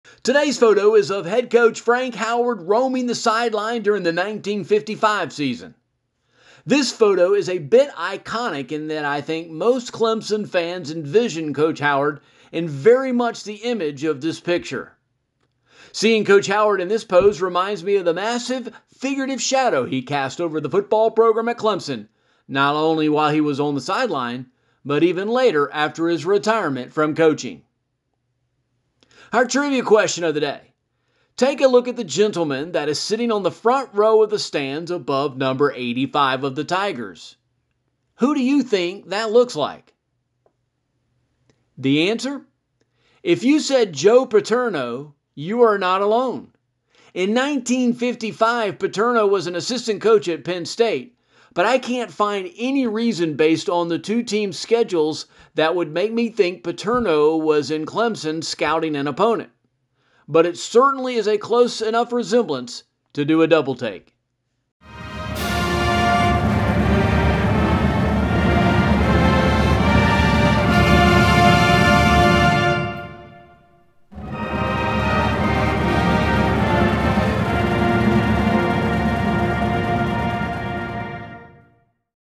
July 3rd Clemson Historic Picture Of The Day
Today’s photo is of head coach Frank Howard roaming the sideline during the 1955 season.

This photo is a bit iconic in that I think most Clemson fans envision Coach Howard in very much the image of this picture.

Seeing Coach Howard in this pose reminds me of the massive figurative shadow he cast over the football program at Clemson, not only while he was on the sideline, but even later after his retirement from coaching

Trivia Question
Take a look at the gentleman that is sitting on the front row of the stands above #85 of the Tigers. Who do you think that looks like?

(Place answer in comments)